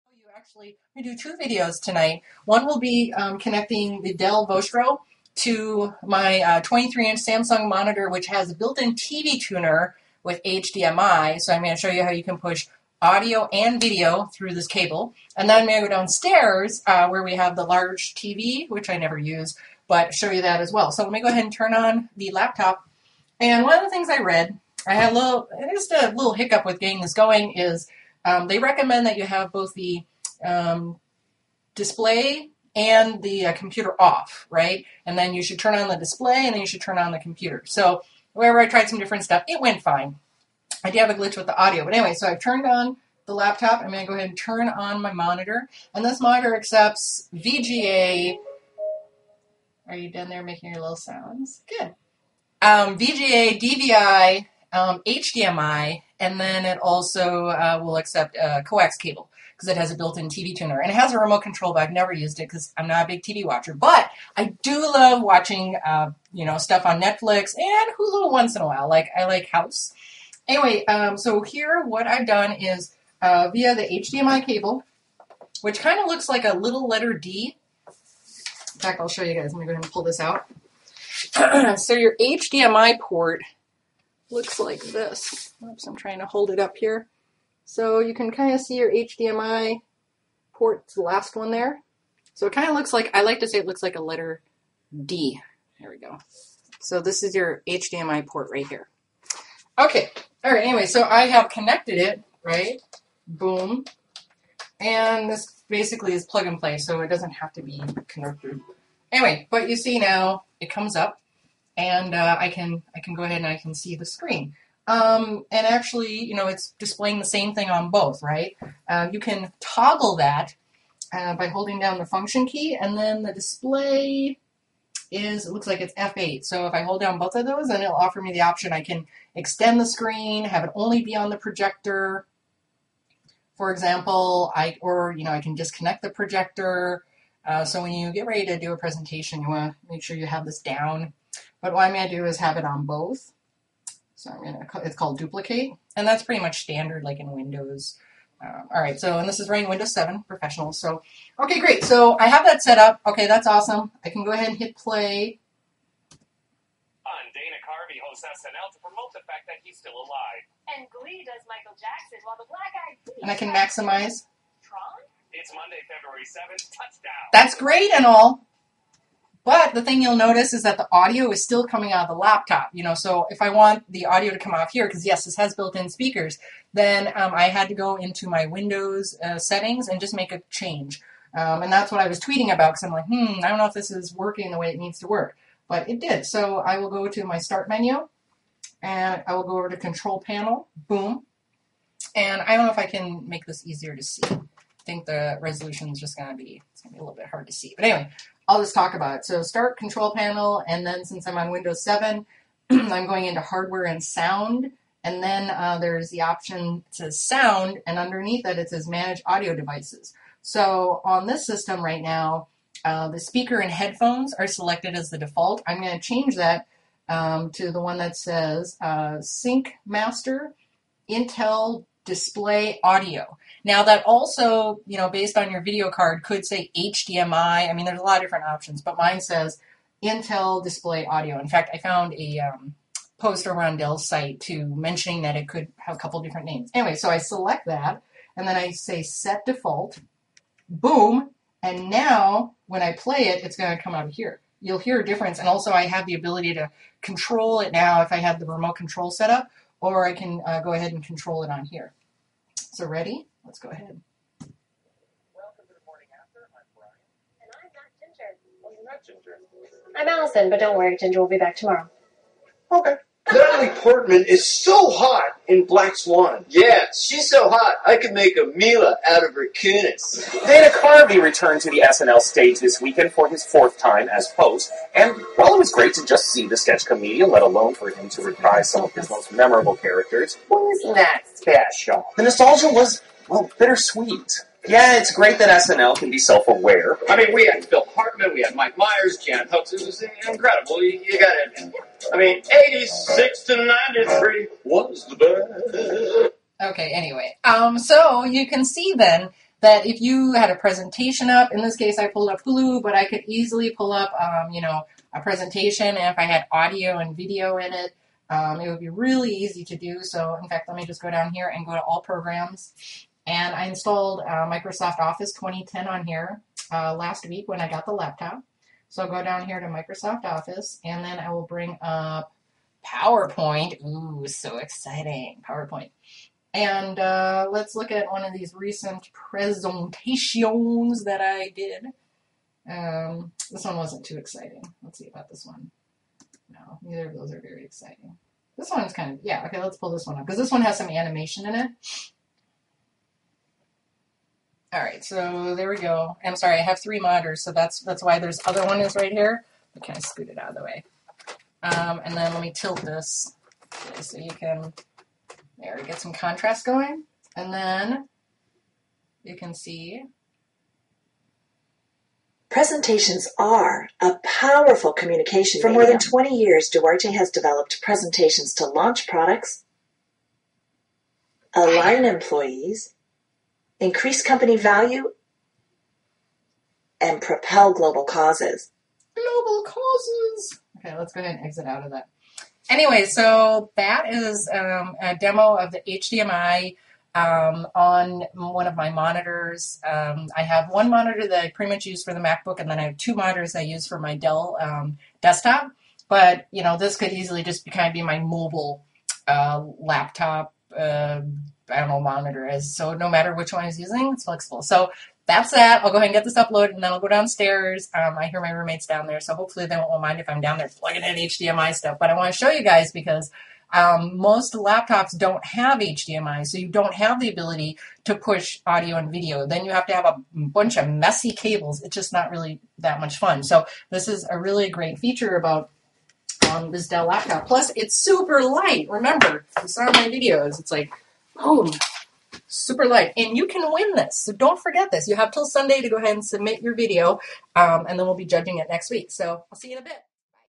How To Connect Laptop to HDMI Flat Screen LCD Monitor
Here I am connecting the Dell Vostro V130 laptop to my 23" Samsung LCD monitor which supports DVI, VGA, HDMI and has a built in tv tuner.

 The laptop is running Windows 7 Professional.

The monitor is my Samsung P2370HD which is a 23" 1080p HDTV with built in TV Tuner.  I paid about $250 for it over the holidays in 2009.  Amazon link listing it currently at $278

Using the HDMI cable, I am able to push both video and audio to the monitor making this a sweet setup for presentations or watching movies.

I'm running a contest giveaway for the laptop to make sure to enter before February 20th, 2011

More general description
I am showing how to connect the Dell Vostro V130 laptop to an external flat screen monitor via the HDMI cable